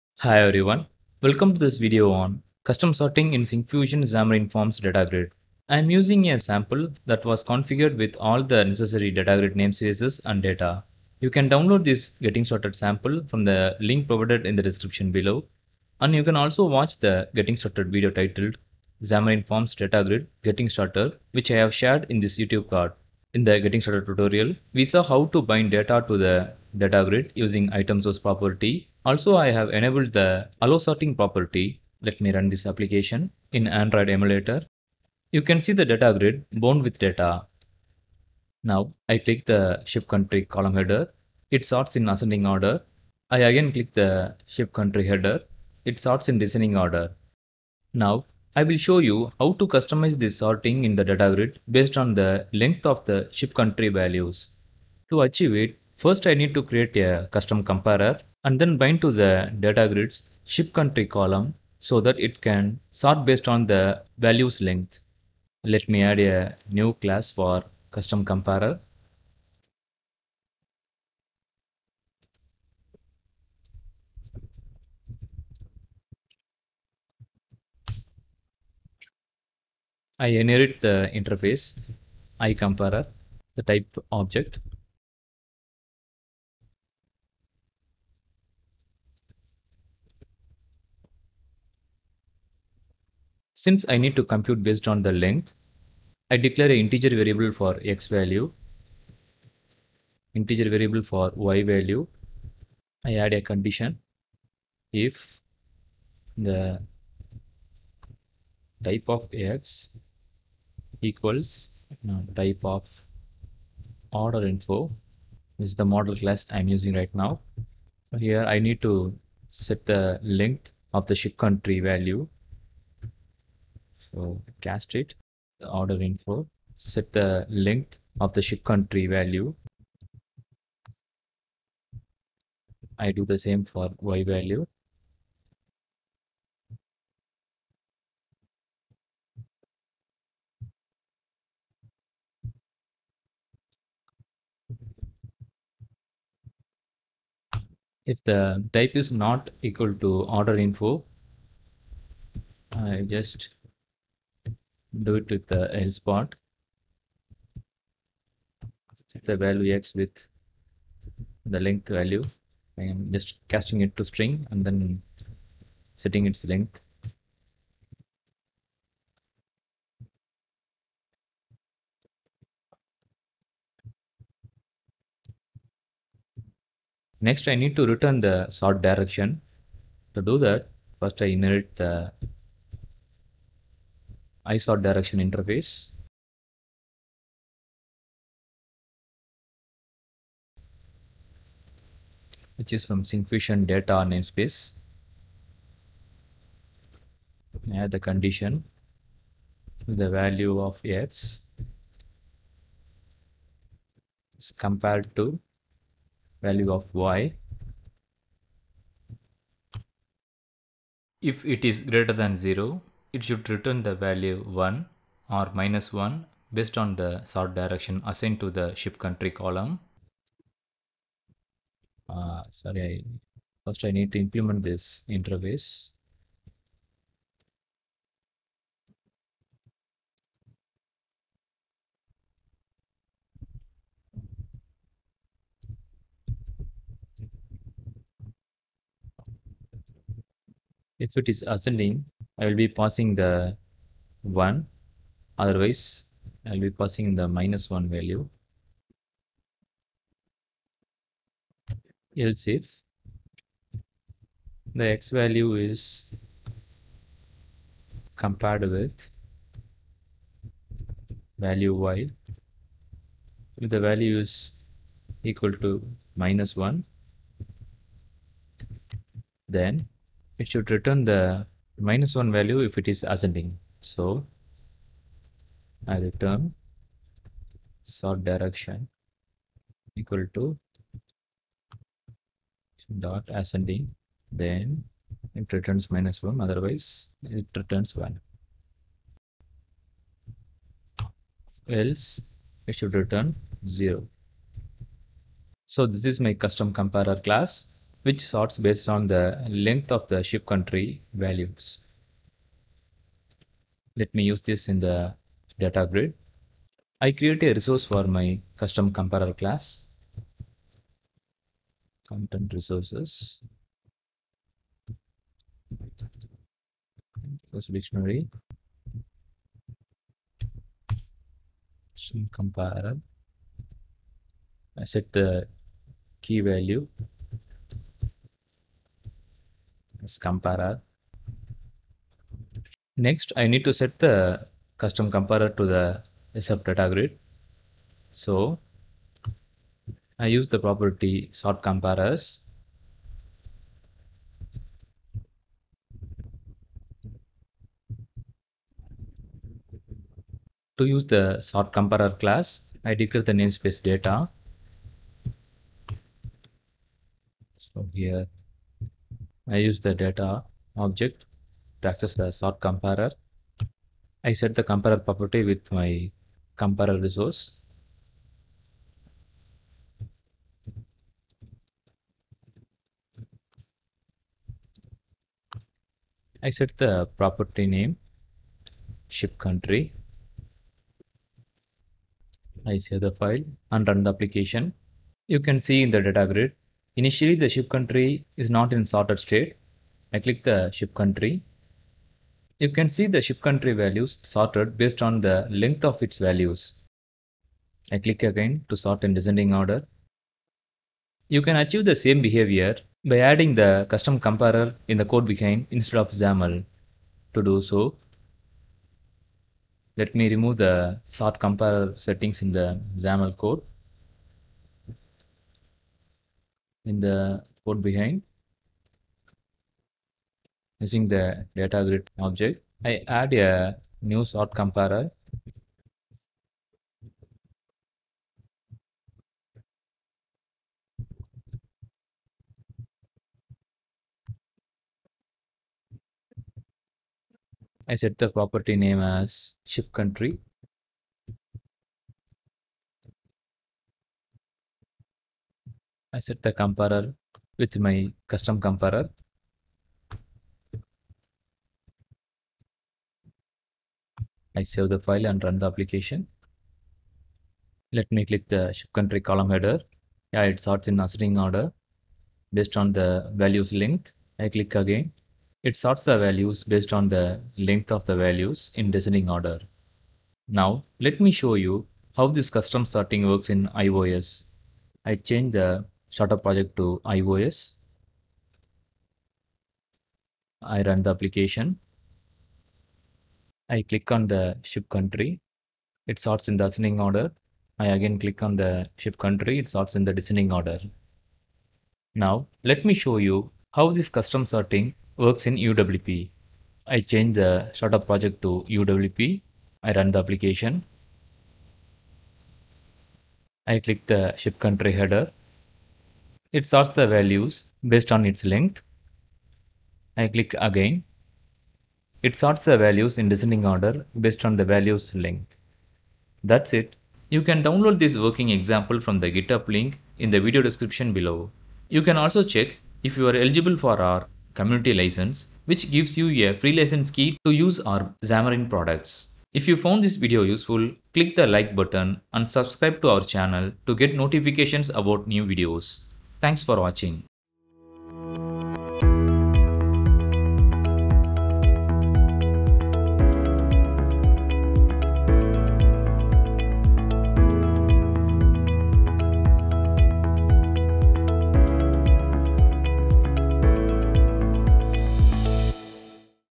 Perform Custom Sorting in Xamarin Forms DataGrid
Learn how to perform custom sort operations in the Syncfusion Xamarin.Forms DataGrid component easily. This video explains how to perform custom sorting in a DataGrid in XAML and in code behind.

[00:00] Introduction
[00:47] Creating custom comparer
[05:31] Set via XAML
[06:40] Set via code behind